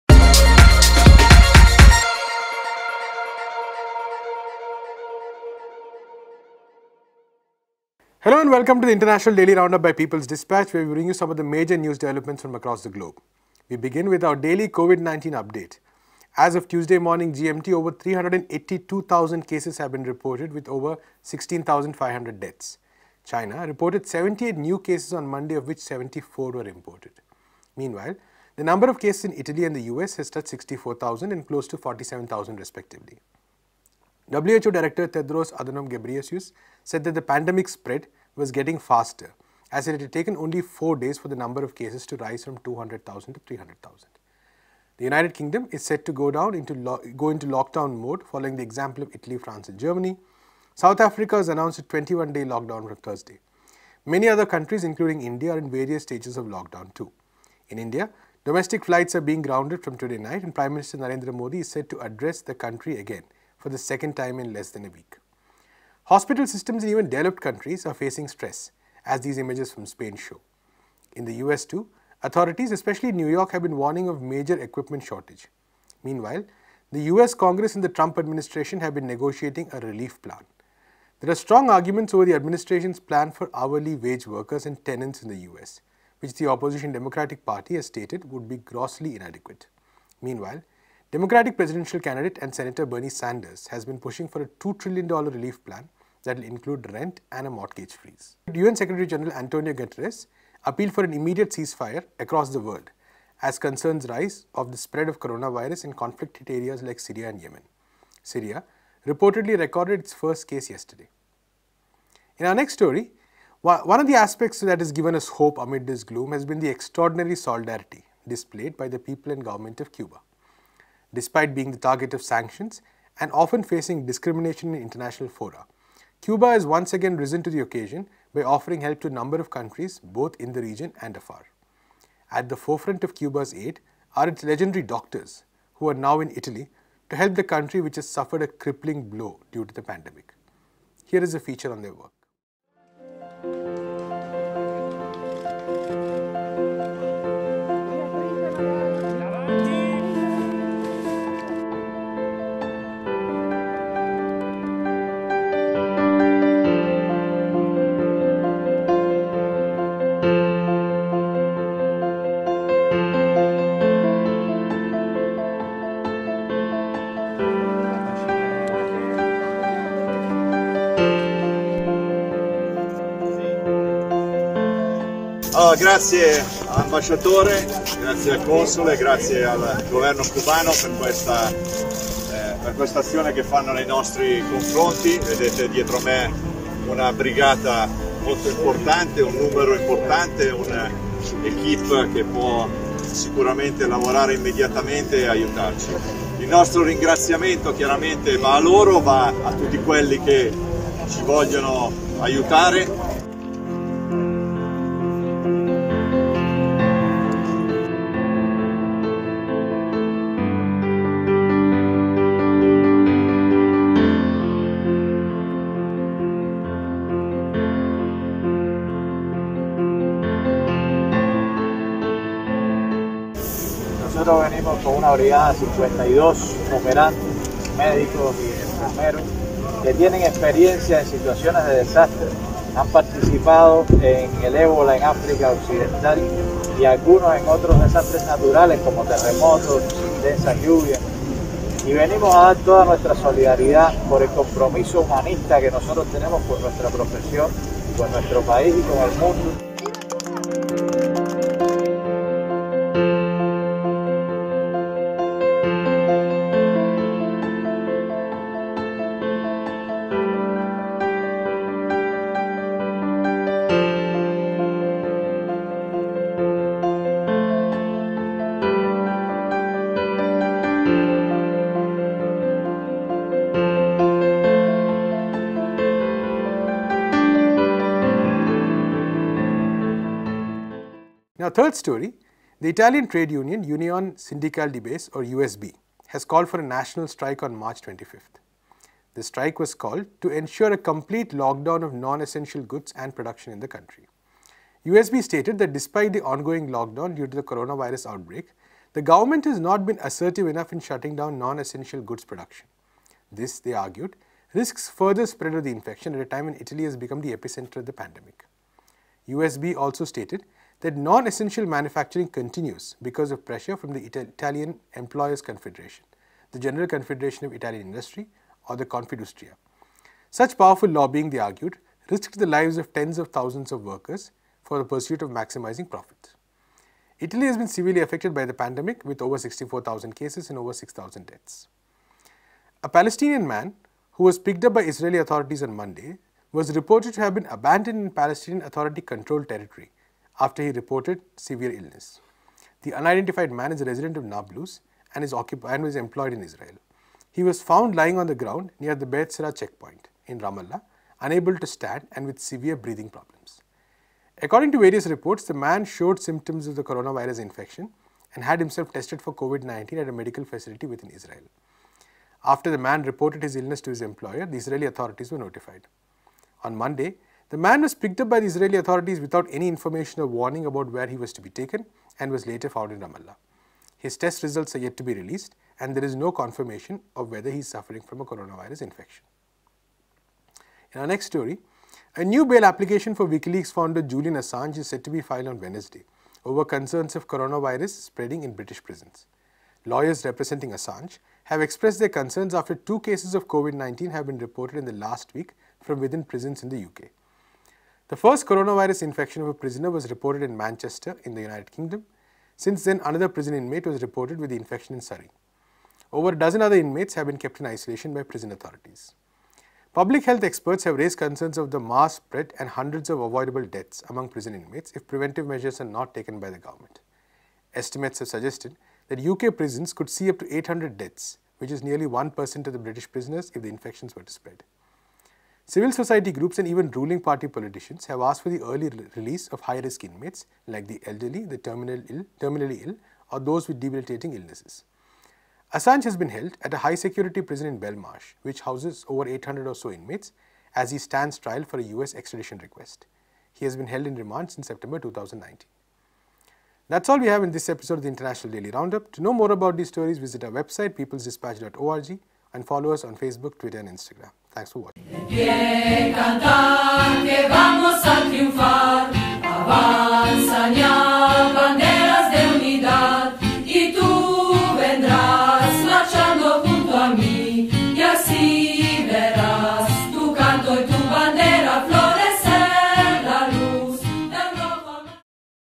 The world comes to a halt over Coronavirus
As over 180,000 cases have been added in less than a week, the coronavirus pandemic is spreading faster, says WHO director. In Italy trade unions call for a national strike of workers in non-essential goods manufacturing, and other stories from around the world.